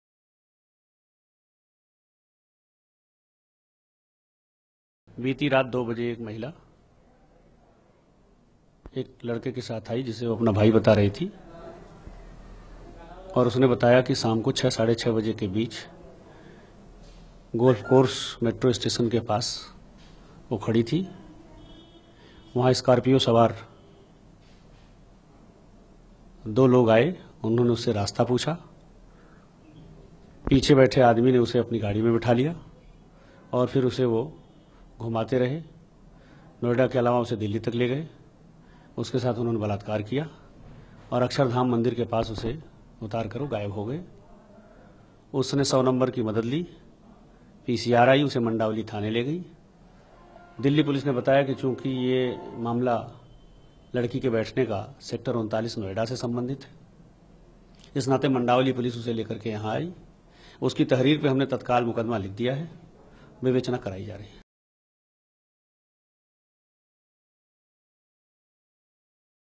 Woman allegedly gangraped in moving car, investigation underway - ANI News
Noida (Uttar Pradesh), Sep 23 (ANI): A woman was allegedly gangraped in a moving car in Noida on late Friday night. The girl was forced into a car by two men who took her to Delhi and raped her. They later dropped her at Akshardham Temple. The girl called police who helped her to the Mandawali Police station. Further investigation in this case is underway.